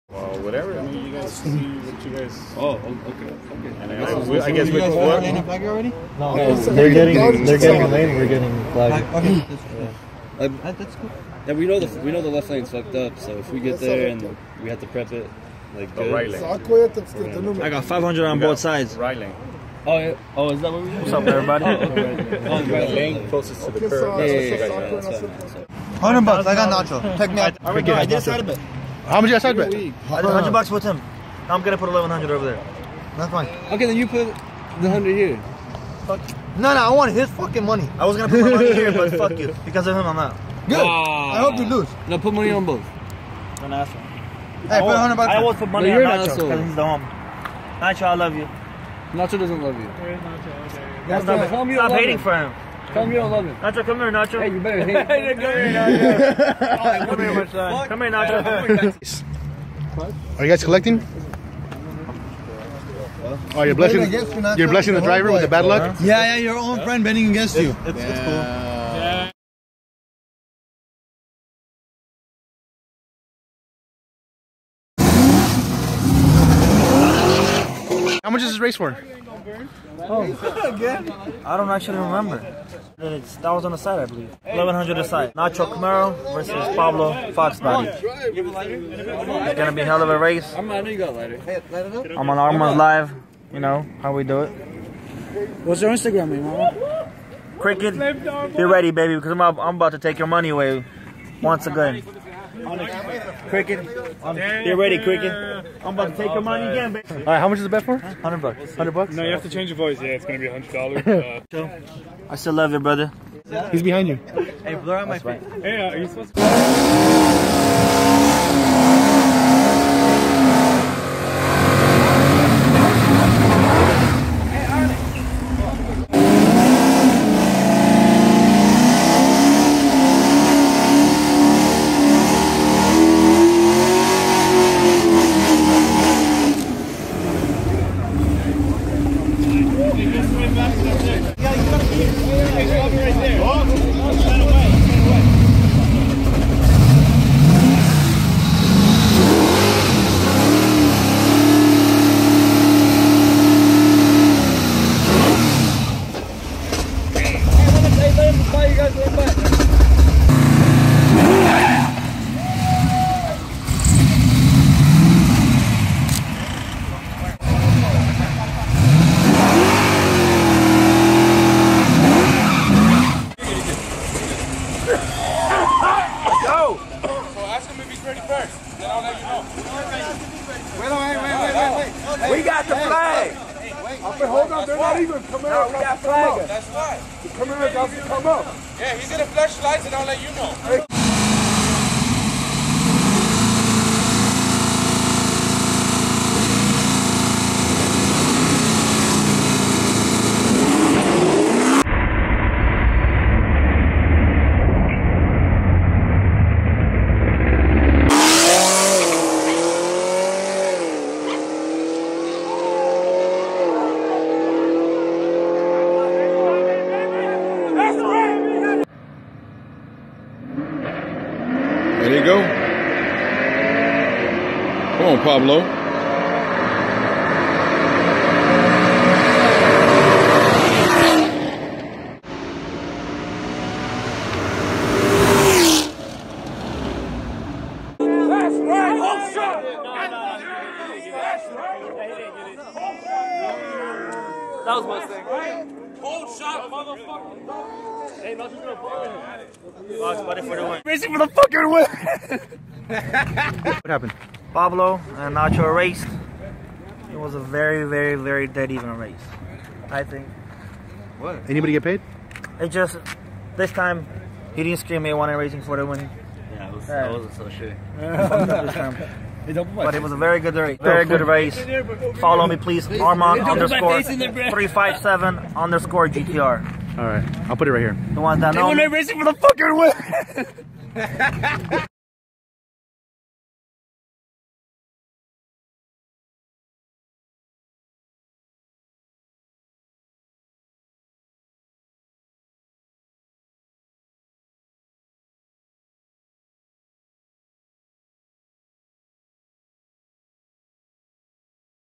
818 Racing Camaro Vs HoleShot Mustang $2,200 Street Race
818 Racing 6th Gen All Motor Camaro
vs
Team HoleShot Mustang

nacho vs pablo

htfu
$2,200

compton speedway